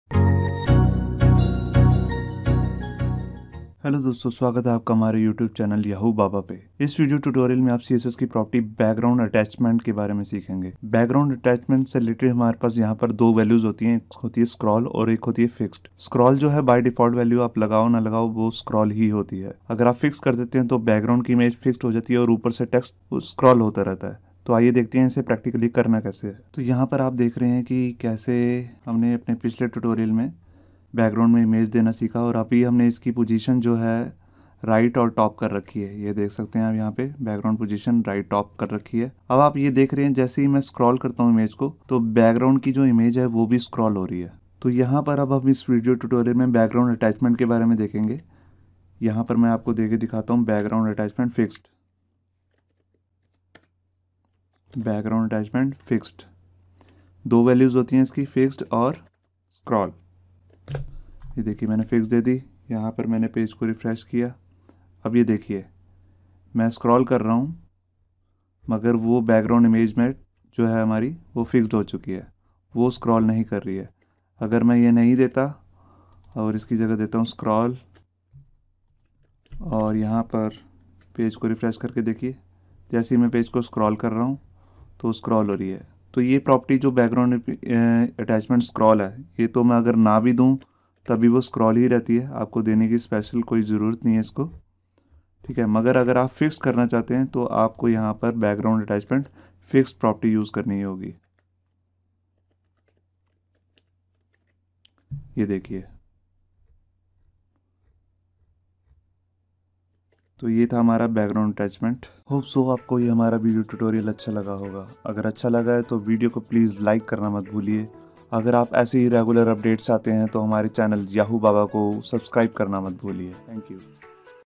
CSS Background-Attachment Tutorial in Hindi / Urdu
In this tutorial you are going to learn css background-attachment in hindi, urdu language.You can learn how to fix the background image while scrolling.